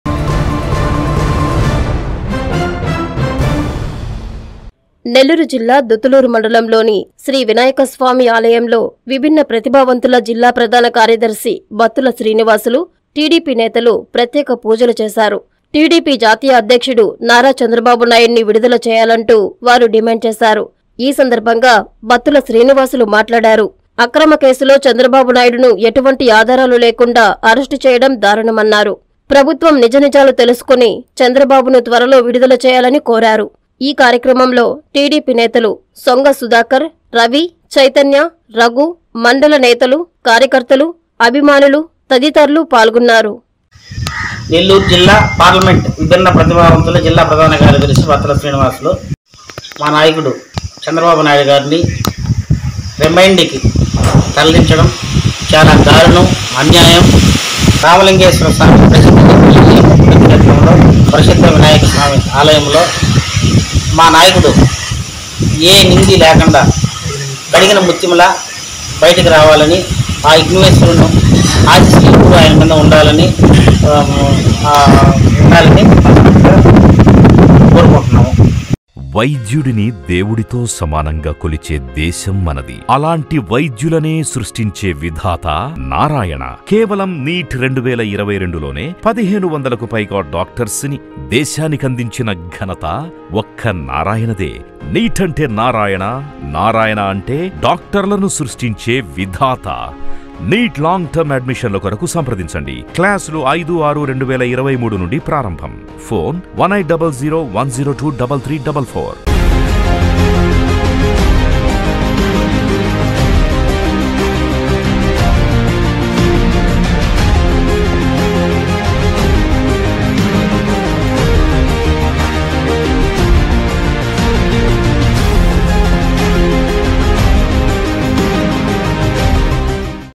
బాబుతో మేము అంటున్న దుత్తలూరు టీడీపీ నేత‌లు
- చంద్రబాబుపై అక్రమ కేసు, అరెస్టుకు నిర‌స‌న‌
- దుత్తలూరు వినాయక స్వామి ఆలయంలో ప్రత్యేక పూజలు